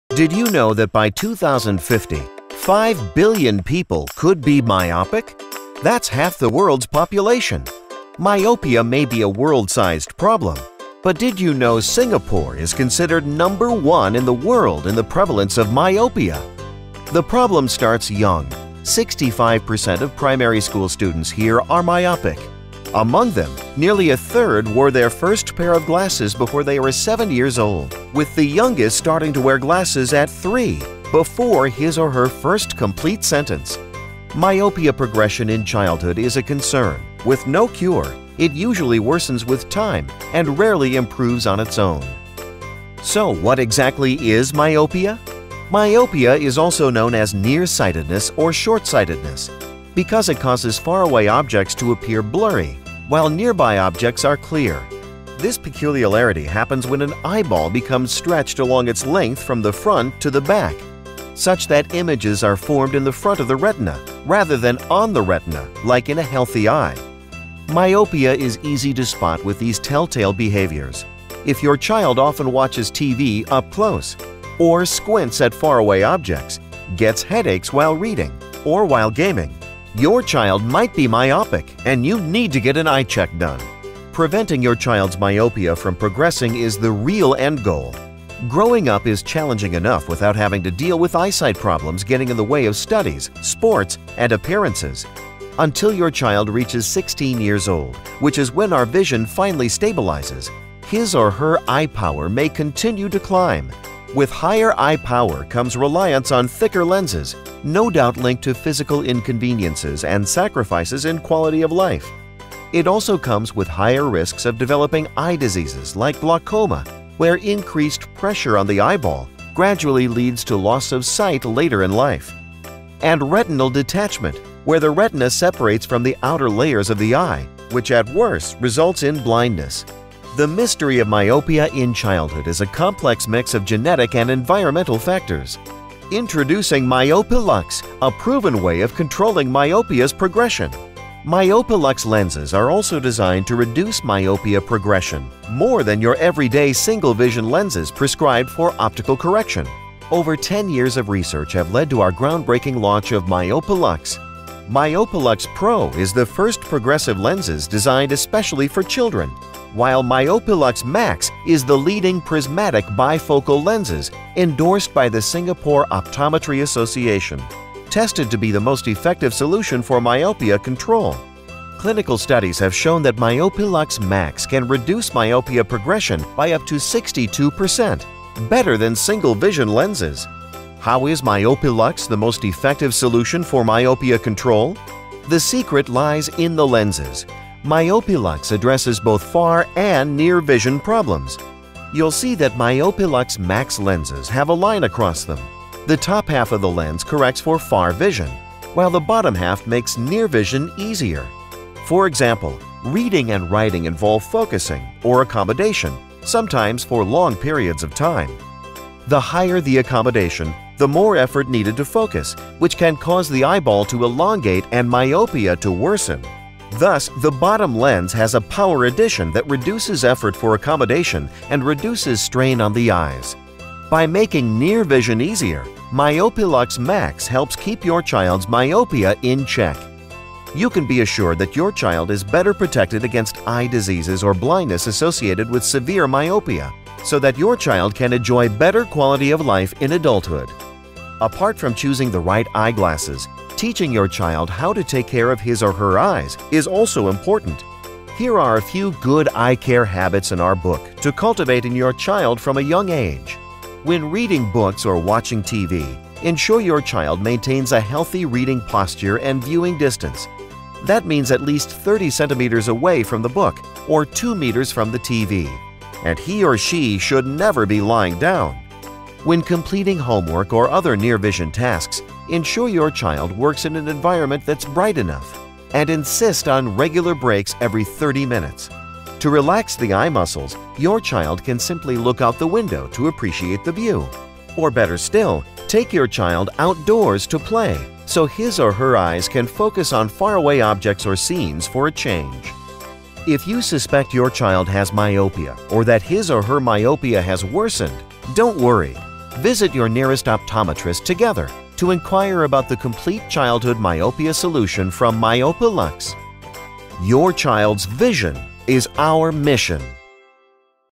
Essilor - Myopilux educational video
An animated video that will take you on an educational trip in the world of childhood myopia and Myopilux.

Agency: Section
Illustration: Section
Animation: Critica